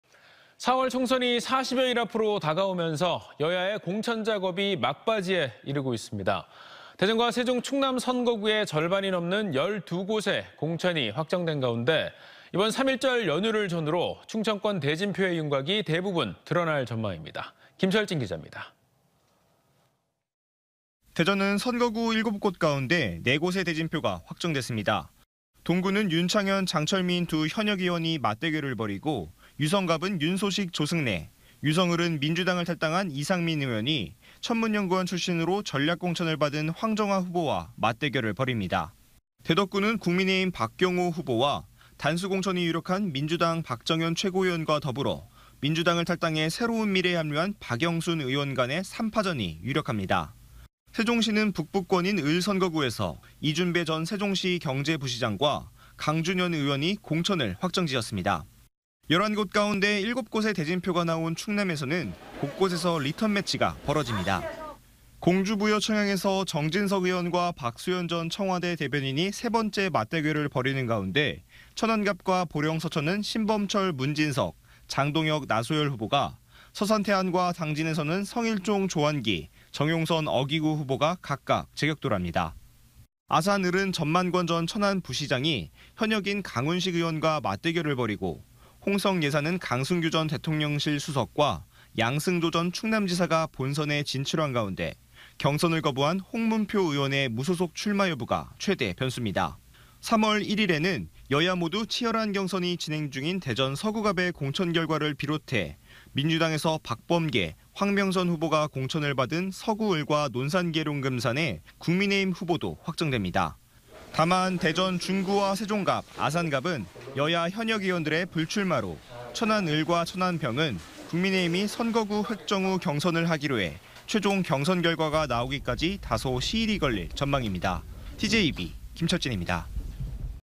총선 공천 막바지..20곳 중 12곳 대진표 확정｜ TJB 대전·세종·충남뉴스
【 앵커멘트 】

4월 총선이
40여일 앞으로 다가오면서
여야의 공천 작업이
막바지에 이르렀습니다.

대전과 세종, 충남 선거구의
절반이 넘는 12곳의 공천이 확정된 가운데,
3.1절 연휴를 전후로 충청권 대진표의
윤곽이 대부분 드러날 전망입니다.

김철진 기자입니다.